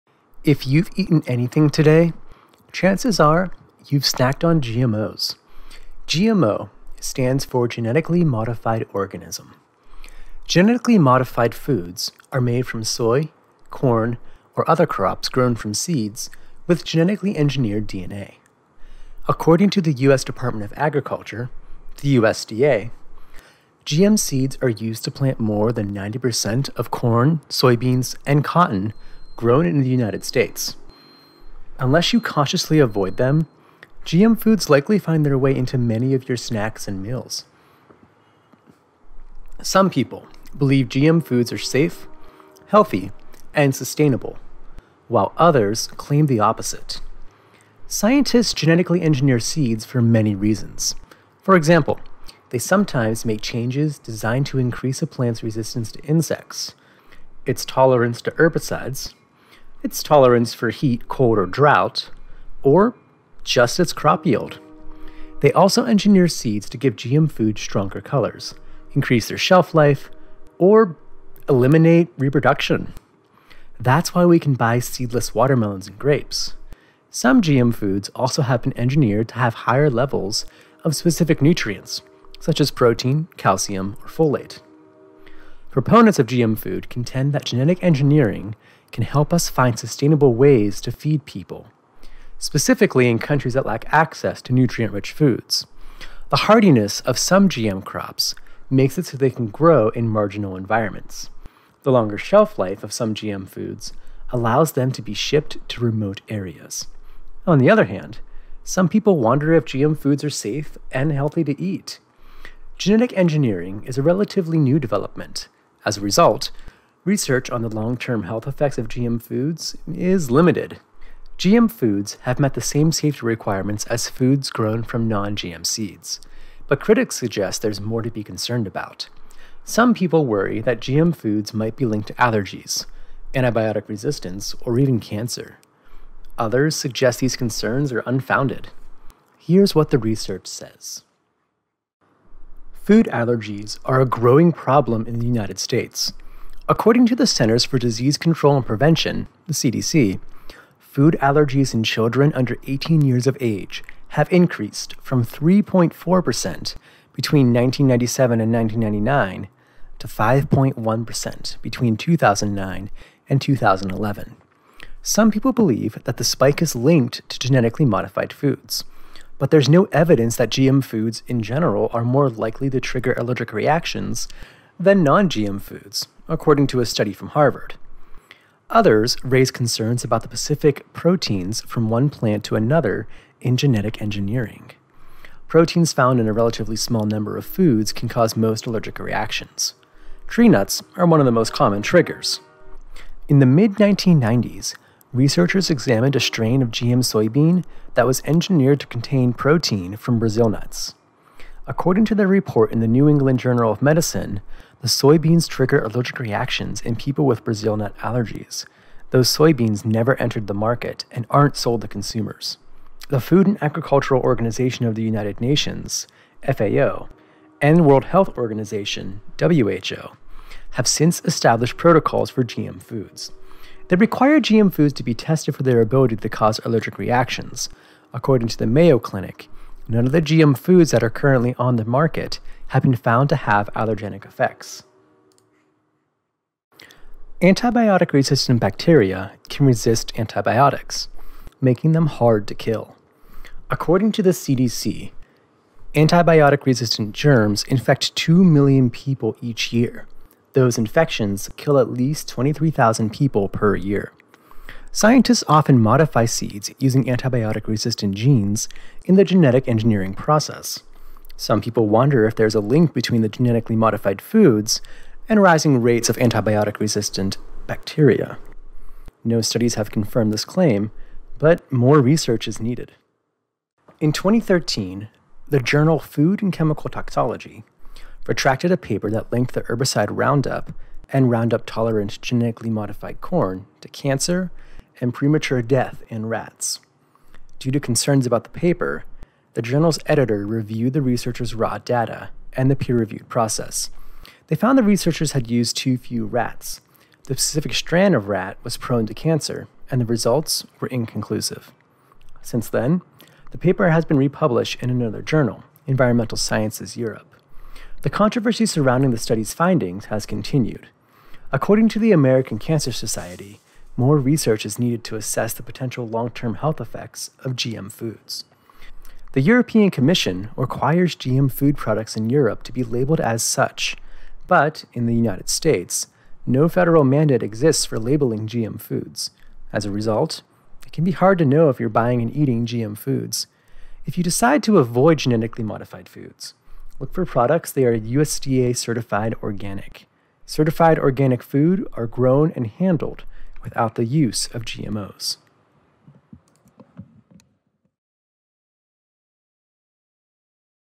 Genetically Modified Seeds Pros and Cons E-Learning Video
HOW MANY GMOS ARE IN AGRICULTURE?
According to the US Department of Agriculture, crop producers use genetically modified seeds to plant more than 90% of corn, soybeans, and cotton in the US. Unless you consciously avoid them, GM foods likely find their way into many of your snacks and meals. Some people believe GM foods are safe, healthy, and sustainable. Yet others claim the opposite.

WHY DO WE NEED GENETICALLY MODIFIED SEEDS?
Scientists genetically engineer seeds for many reasons. For example, they sometimes make changes to increase a plant's resistance to insects, its tolerance to herbicides, its tolerance for heat, drought, or crop yield. They also engineer seeds to give GM foods stronger, colors, increase their shelf life, or eliminate reproduction. That's why we can buy watermelons and grapes both with or without seeds. Additionally, some producers engineer GM foods to have higher levels of specific nutrients, such as protein, calcium, folate. Supporters of GM foods find genetic engineering a positive advance in technology.  They argue that GM foods help to solve the world's hunger crisis. New GM food can help scientists create ways to feed people in countries that lack access to nutrient-rich foods. One big benefit is the hardiness of some GM crops. They can often grow in marginal environments. GM food can also last longer on the shelf.  This longer shelf life allows remote areas to have access to these fresh foods.

This video will take a look at the pros and cons of GM seeds.

Support Ocean Clean Up with 4oceans!

Did you know:
- Genetically modified seeds are used to plant more than 90 percent of corn, soybeans, and cotton grown in the United States.
- Scientists genetically engineer seeds for many reasons.
    - resistance to insects
    - tolerance to herbicides
    - tolerance for heat, cold, or drought
    - crop yield
    - stronger colors
    - increase their shelf life, or eliminate seeds

There are many Pros and Cons of modified foods.

References:
- US Department of Agriculture
- Healthline.com